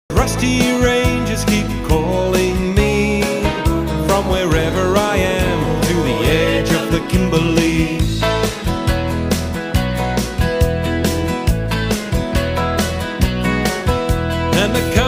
mayriver Meda/derby western Australia.
welcome to Kimberley homestead
sunday 4th Dec 22 we headed out a short drive from derby to mayriver in the stunning kimberley in northernwesternaustralia.